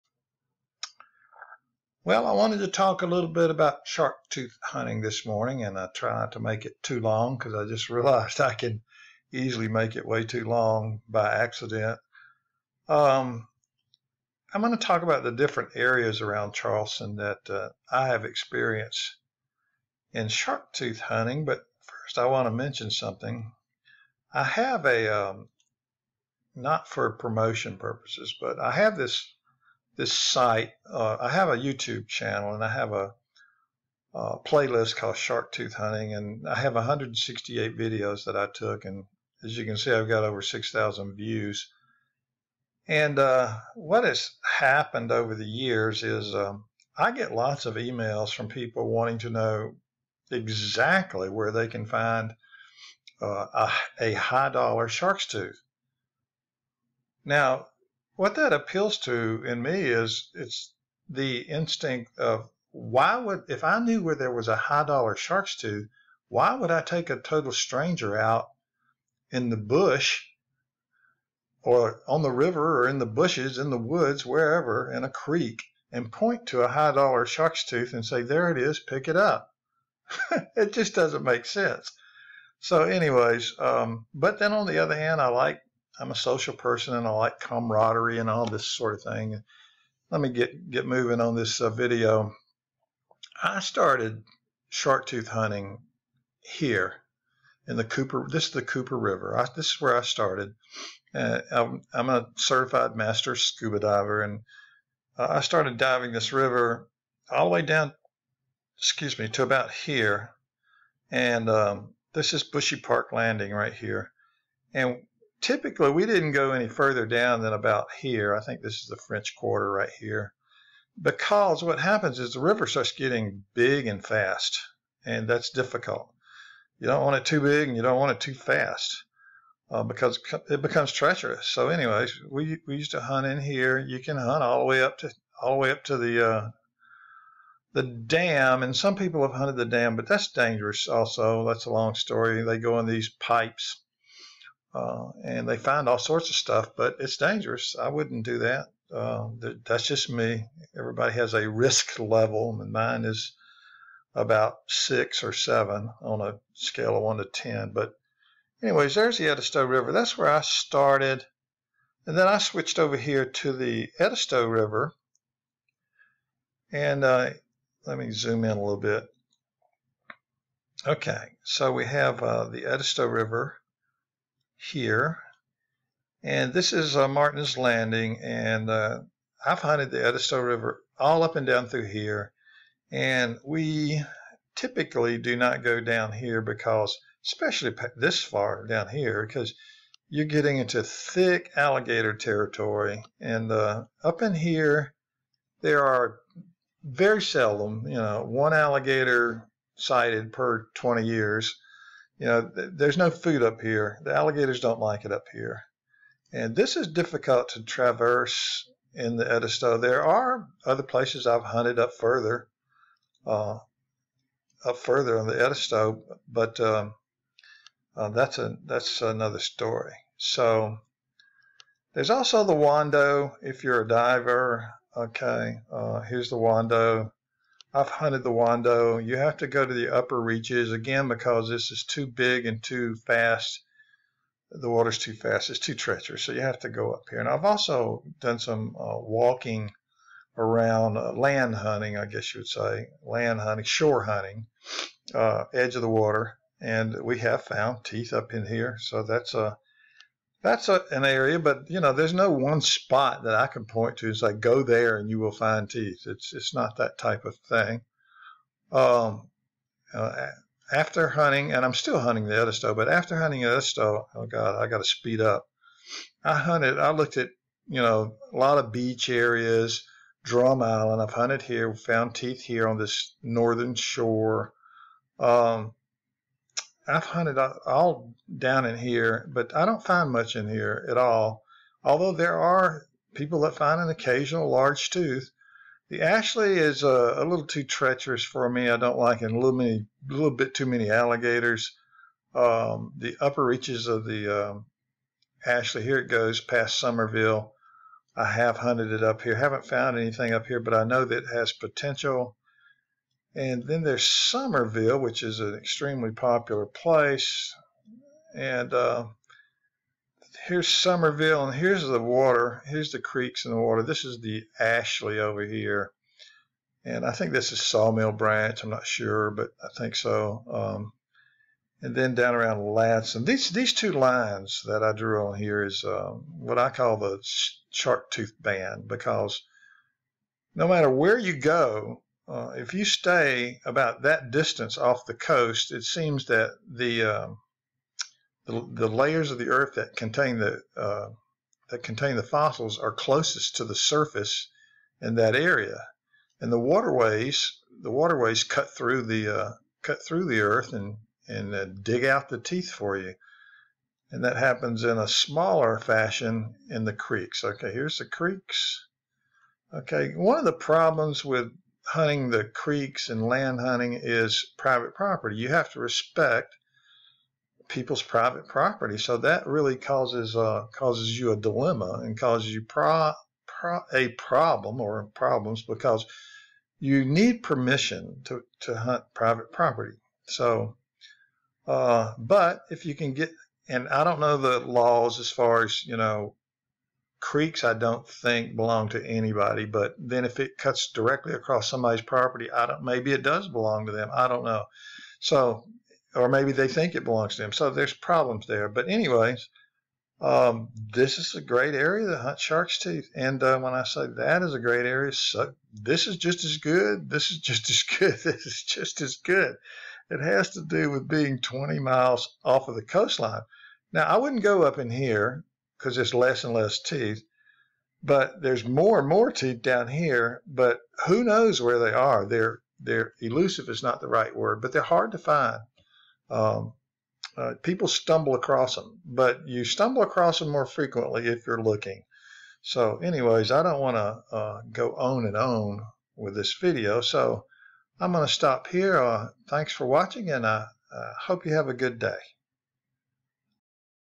Hunting for fossilized sharks teeth in the Lowcountry of South Carolina
In this video I use Google Maps to talk about my history of hunting for fossils (primarily sharks teeth) in and around Charleston SC. Reference Cooper River. Edisto River. Wando River. Summerville creeks. In the end, your best bet is to use Fossil Hunting Guides. Yes there is a cost but it eliminates a lot of otherwise problems

Refrence:  Fossils are formed in the sedimentary layers of the earths crust. They will remain there millions of years until something disturbs them. Rain, rivers, and creeks are what typically cause a fossil to become dislodged from the sediment. The fossil will then gravitate towards other materials of the same weight and density. This is why sharks teeth are often found in gravel beds

I